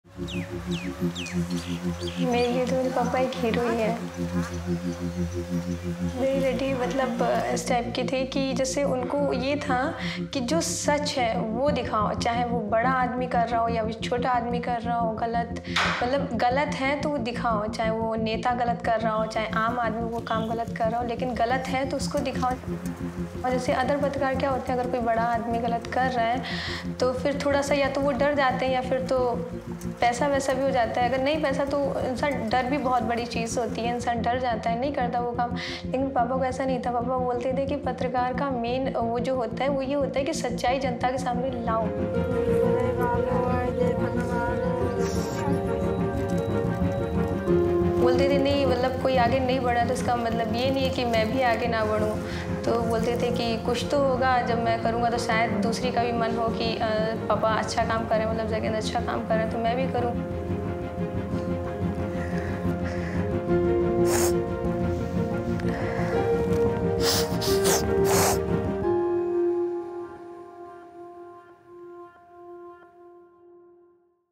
Green Blood - Diksha Singh talks about her late father, Jagendra Singh
Diksha is Jagendra Singh’s daughter, the journalist who was burned alive and died in 2015, after accusing a local official of various acts of corruption. Diksha tells Forbidden Stories who her father was: a journalist fed up with injustice.

The “Green Blood” series pursued the investigations of several reporters who faced threats when covering environmental damage and other abuses by mining companies.

For the first time, 40 journalists from 15 countries organized by Forbidden Stories were able to go in to the field, track supply chains and use open source tools to look into mines in India, Tanzania and Guatemala where questionable practices had previously been covered up.

The results of this eight-month-long collaborative investigation will be published the week of June 17 in 30 media organizations around the world to keep those stories alive.